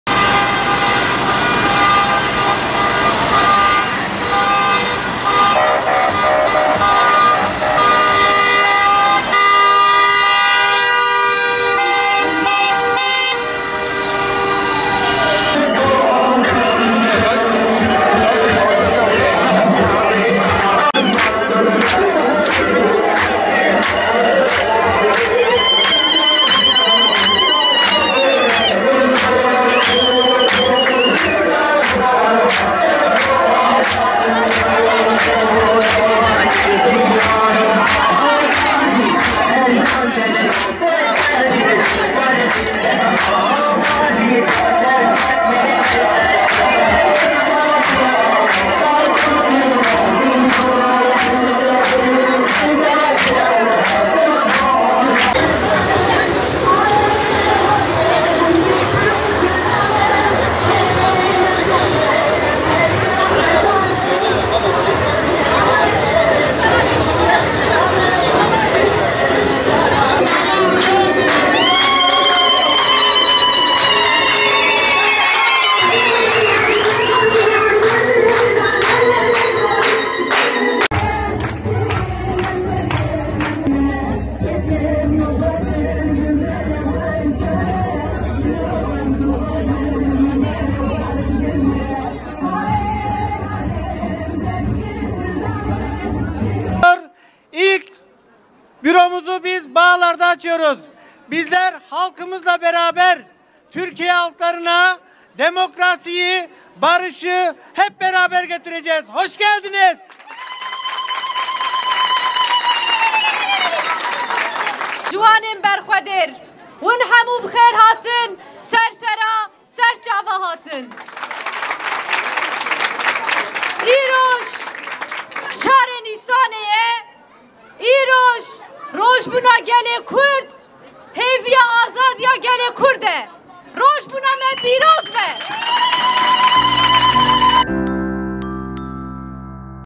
YEŞİL SOL PARTİ'NİN AMED'TEKİ BÜRO AÇILIŞINA YOĞUN İLGİ
Yeşil Sol Parti, Amed’in Rezan ilçesinde ilk seçim büro açılışını halkın yoğun katılımıyla gerçekleştirdi.